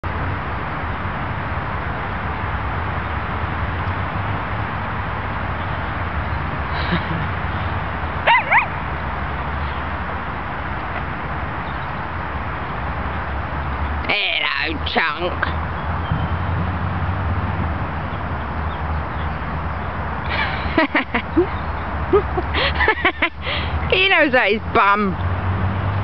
Shocking truth of allowing small dogs near a Bullmastiff
Filmed in real time and unedited, this video shows what could ACTUALLY happen when little dogs are not kept on a tight lead and/or picked up. This might shock some people.